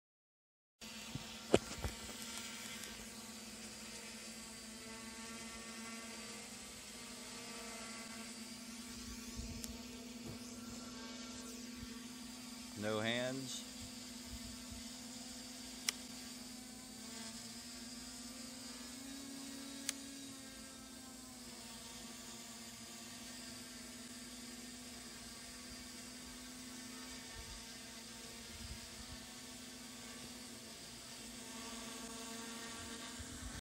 Quadcopter-s500 with retracts--Gps lock and altitude hold
Naza GPS lock and altitude hold V2 steady in the wind.  Filming with my smartphone while flying at the same time.  Of course Pinky was there to co-pilot.  Thanks Pinky.